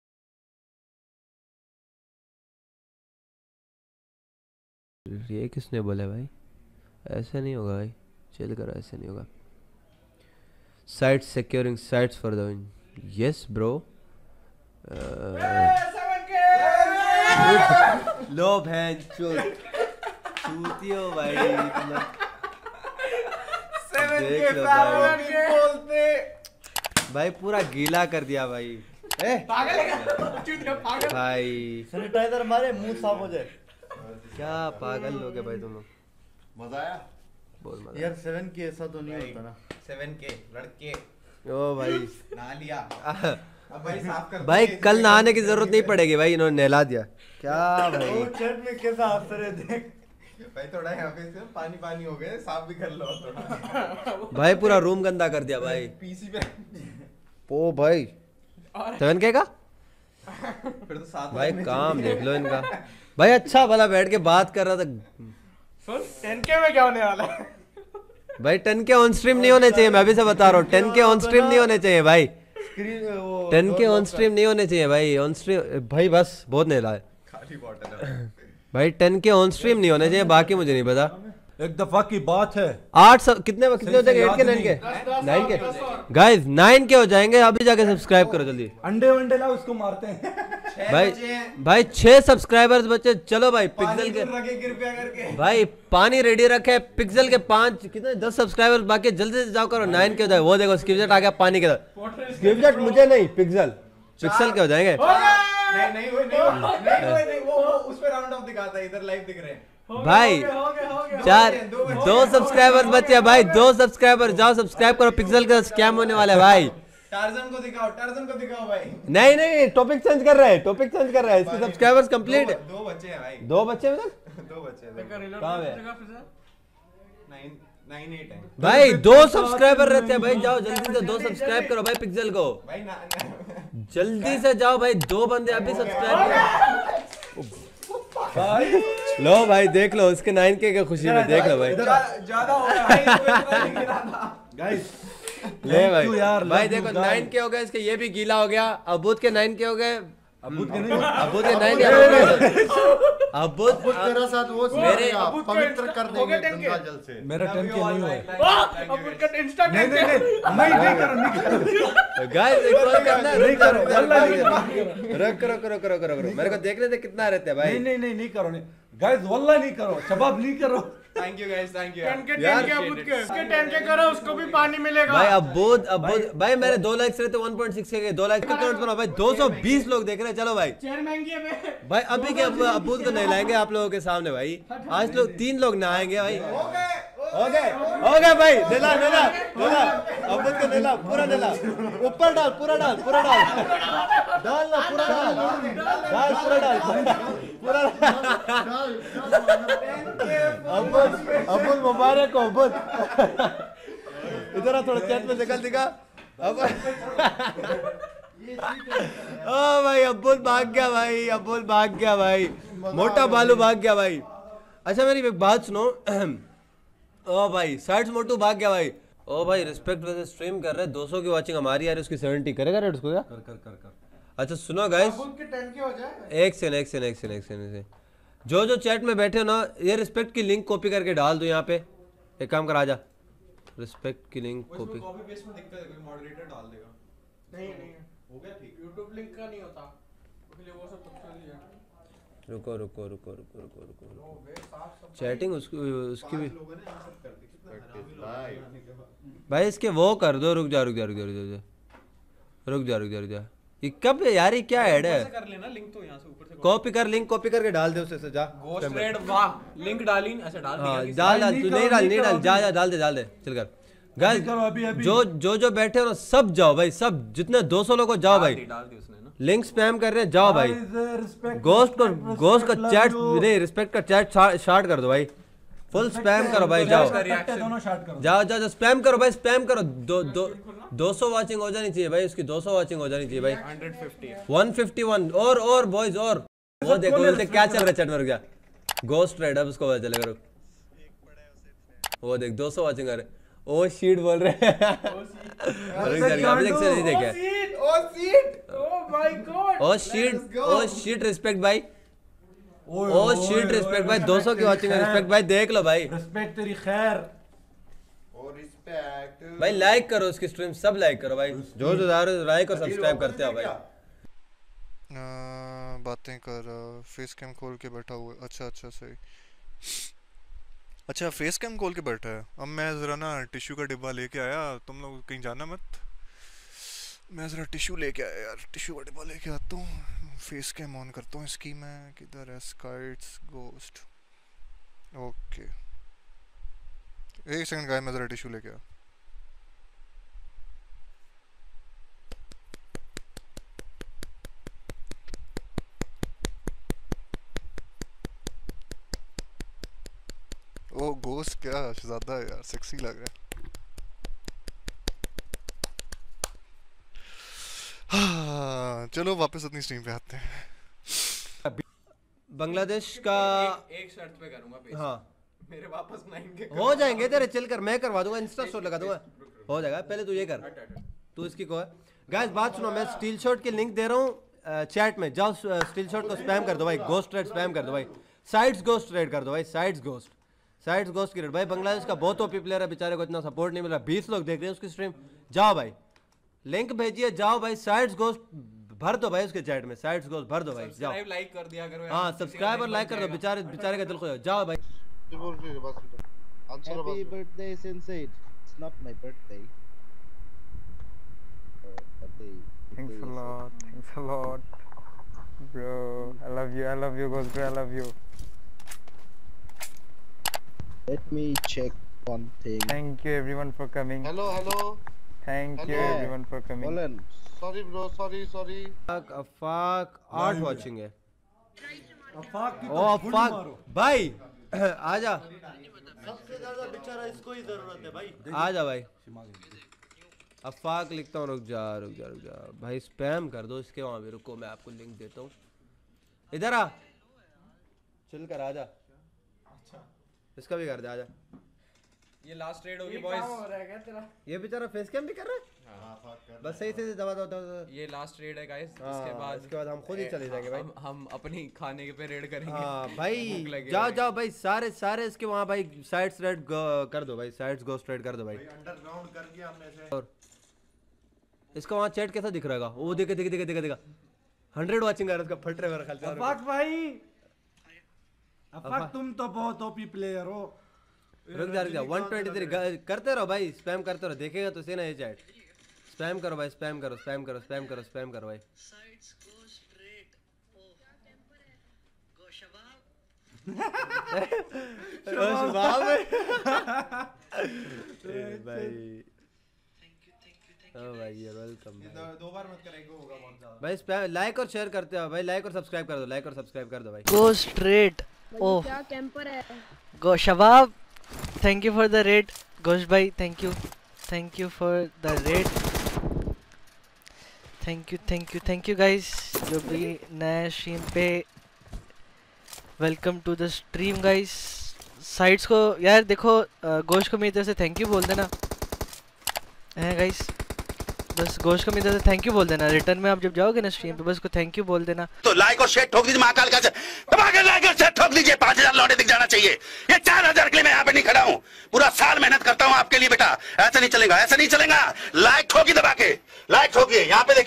SCYTES GHOOST RAID ON STEELSHOT , FSRESPEKT , AFFAQ | TEAM SCYTES BOOTCAMP
SCYTESGHOOST RAID ON FS RESPEKT , AFFAQ & VENOM STEELSHOT
FUNNY HIGHLIGHTS OF TEAM SCYTES BOOTCAMP | FS RESPEKT REACTION ON SCYTES GHOOST RAID

VIDEO CREDITS:
SCYTES GHOOST LIVE STREAM :